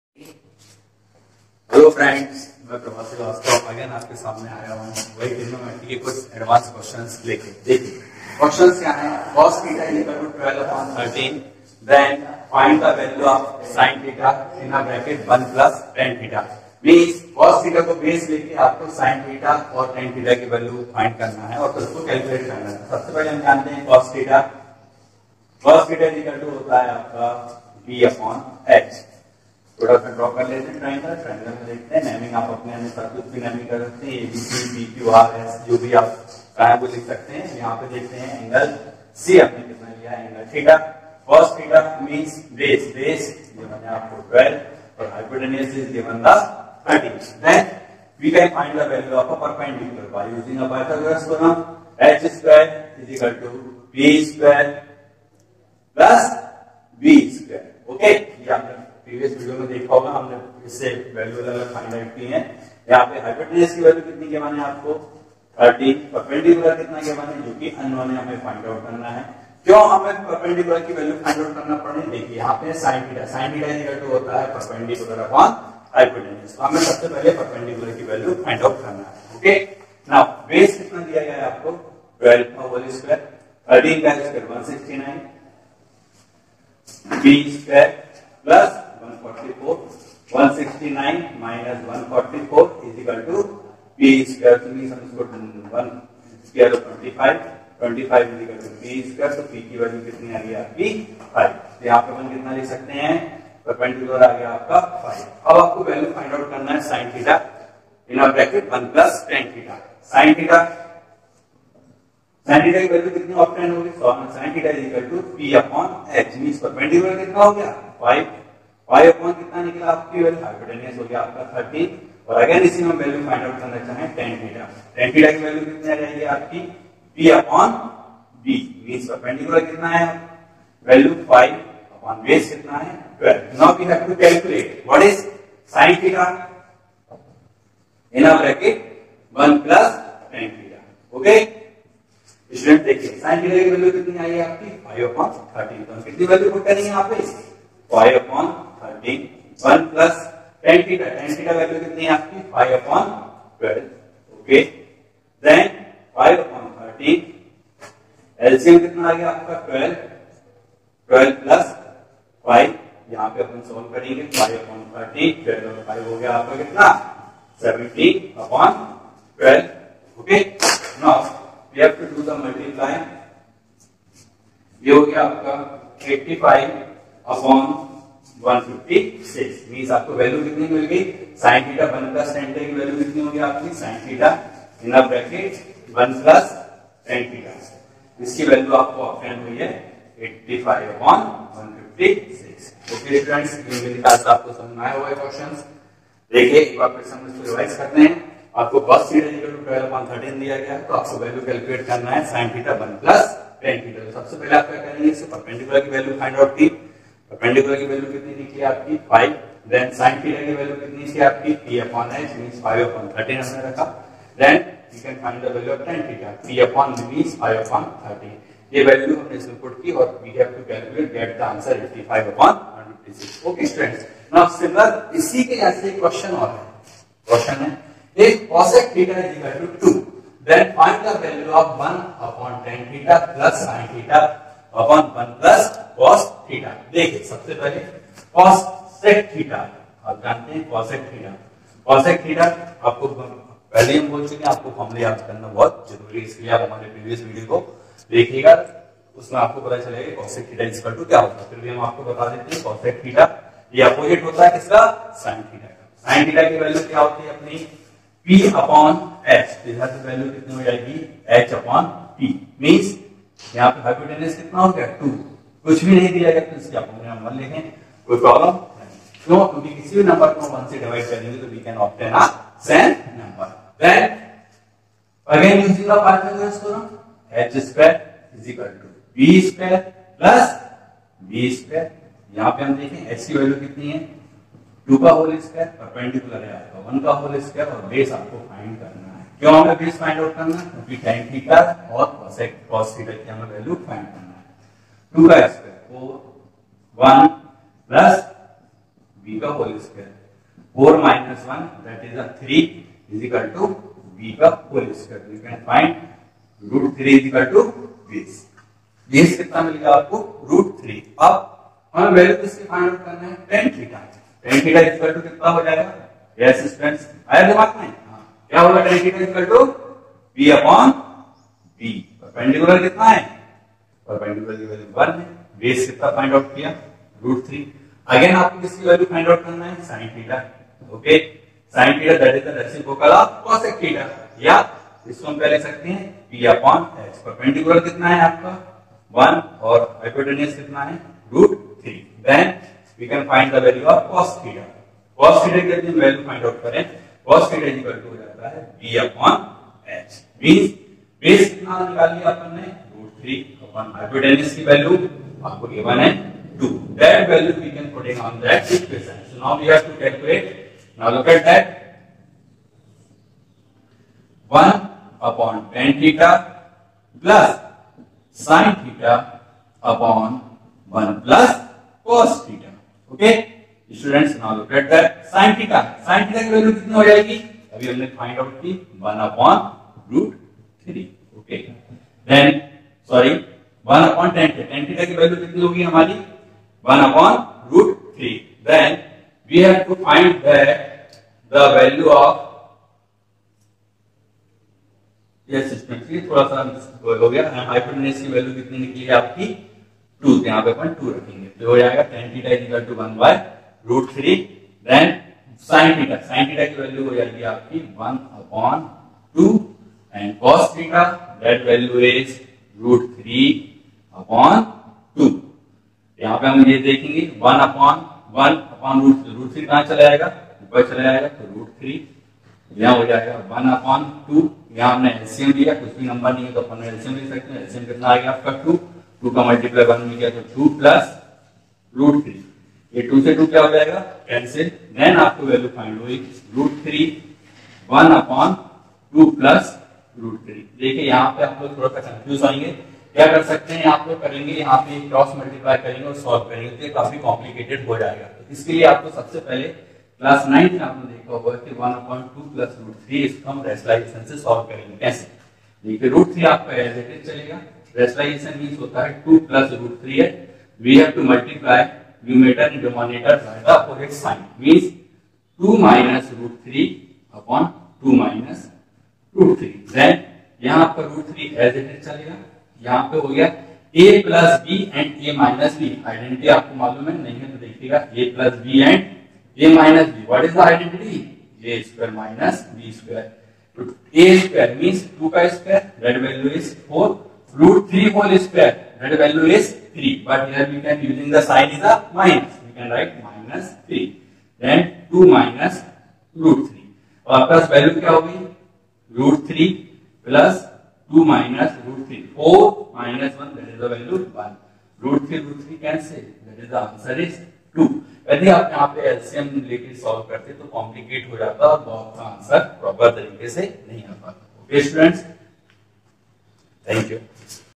Maths/CBSE/ICSE/Mr.Prabhat Shrivastava Sir/part-04
In this video we will discuss some advanced questions based on Tigonometric formulas...
But before this you mast have to watch  previous classes..
Thanks